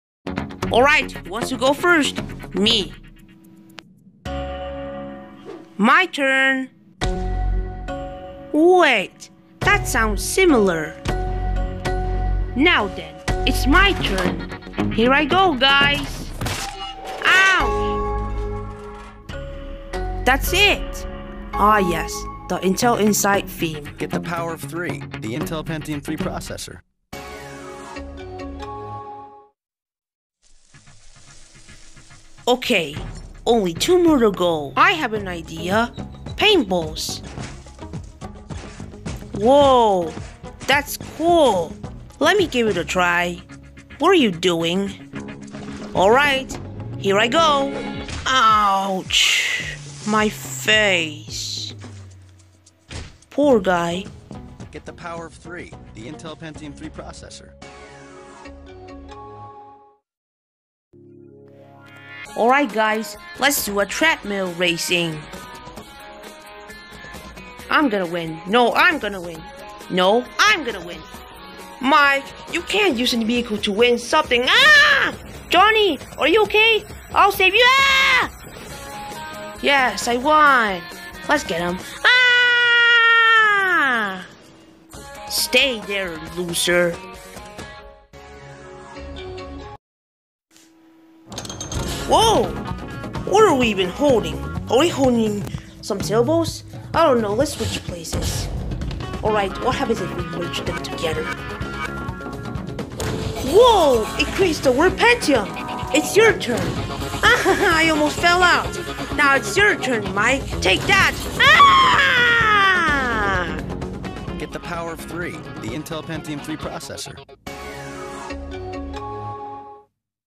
All Intel Pentium 3, 4 and Centrino BMG Commercials, but I dubbed it. (Part 1 of 3/Reupload)
Reuploaded because it was removed.
Seriously, YouTube better not make one false movement again.

BMG = Blue Man Group